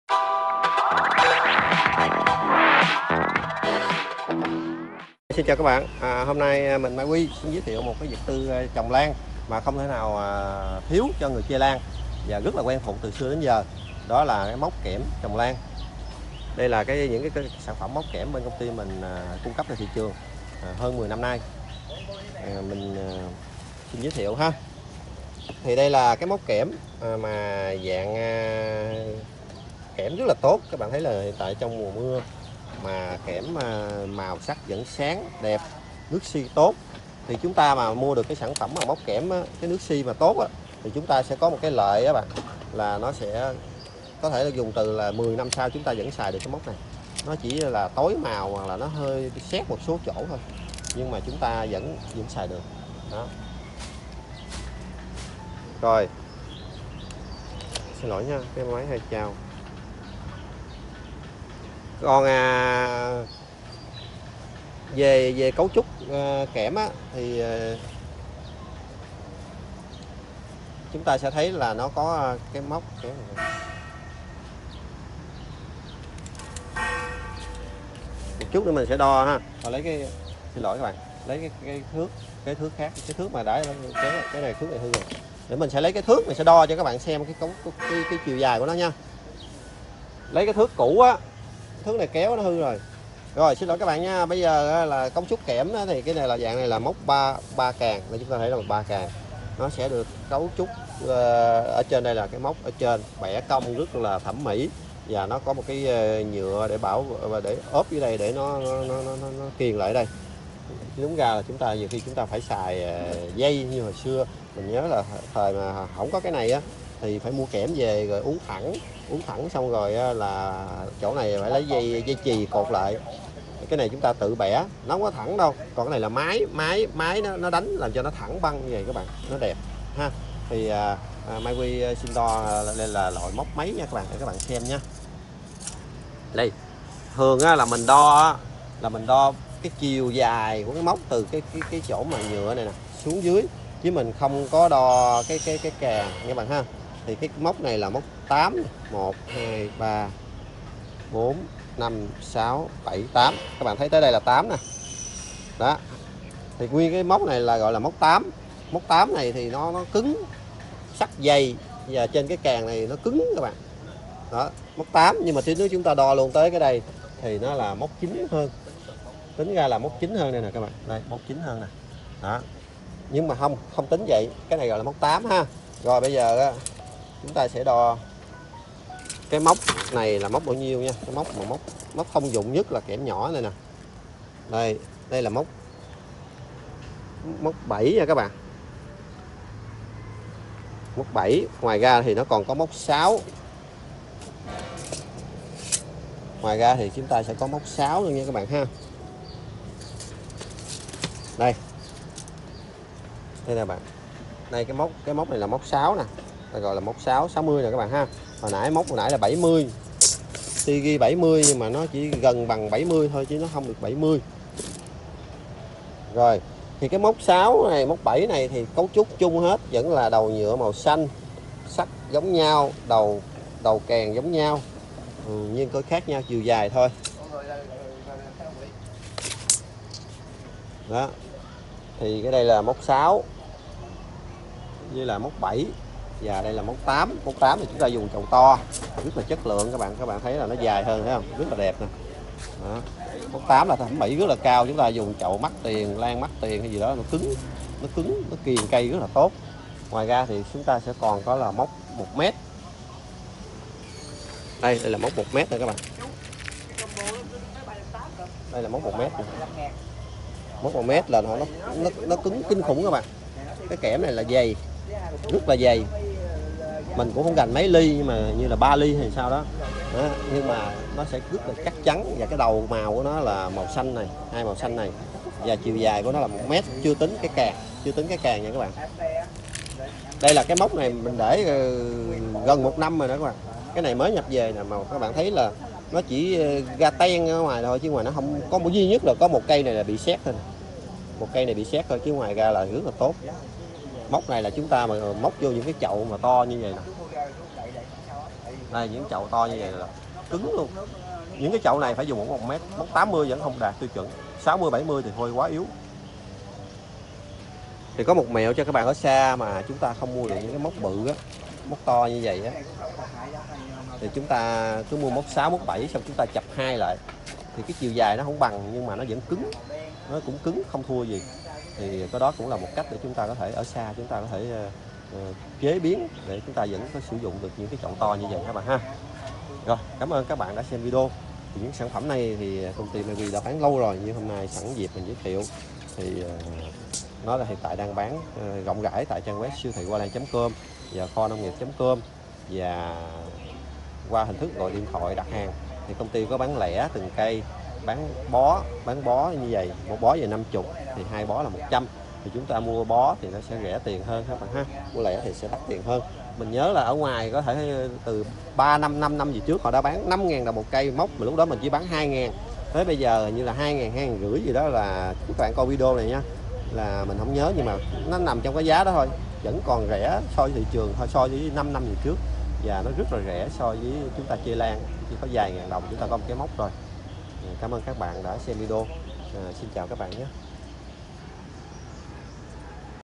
Dụng cụ trồng lan không thể thiếu từ vài chục năm nay là gì? [ KNN ]
Dụng cụ trồng lan không thể thiếu từ vài chục năm nay là gì?

Mô tả: Móc chậu lan: dùng kẽm chất lượng tốt, ít bị rỉ sét, thời gian sử dụng lâu dài.
Móc Đủ Loại
Giá: 2,5k/ Móc thường
7k/ Móc 2 càng
7k/ Móc 3 càng dài 80cm
10k/ Móc 3 càng dài 1m
3,5k/ Móc 3 càng dài 70cm
3k/ móc 3 càng móc ngắn 60cm